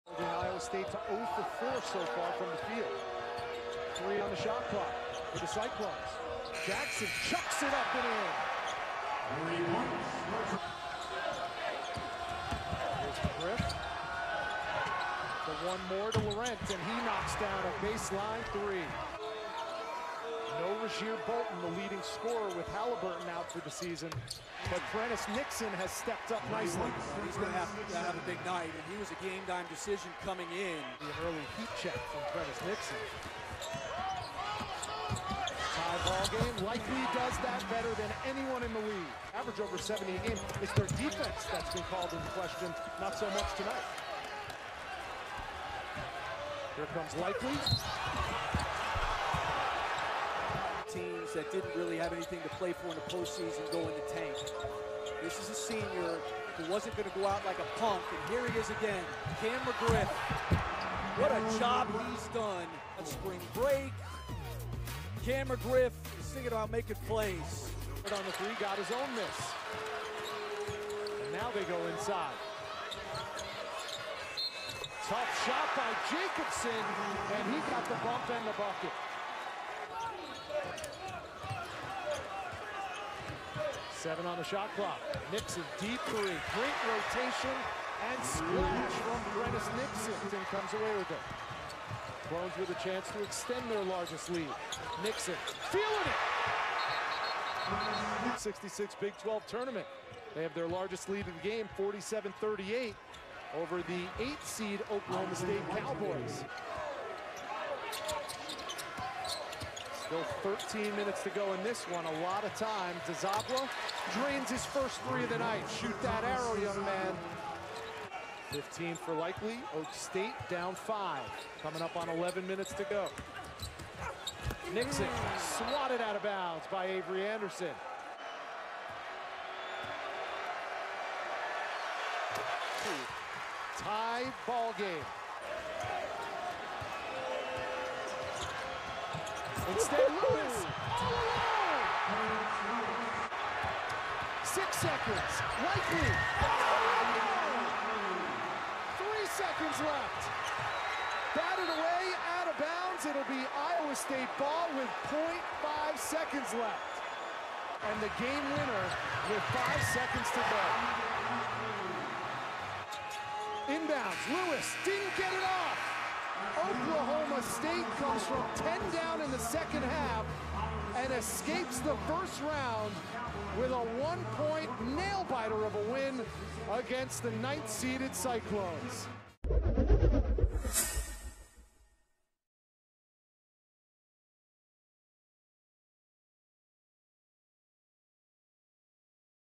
Iowa State vs. Oklahoma State Highlights - Big 12 Men's Basketball Tournament (2020) | Stadium
These teams exchanged blows all night and Iowa State carried a lead late into the second half, but Oklahoma State fought its way back to win by a final score of 72-71. Watch the highlights from the Big 12 Tournament thriller here.